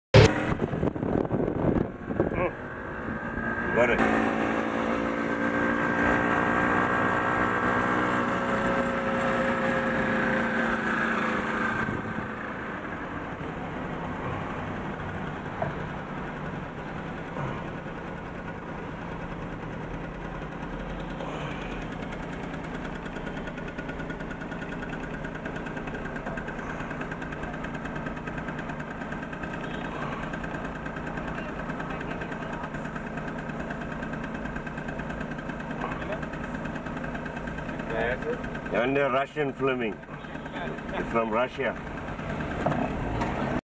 DURBAN - A dog in a helmet
We were driving with my friend Ramesh Lutchman on one of the streets of Durban and suddenly saw strange group on a motorcicle...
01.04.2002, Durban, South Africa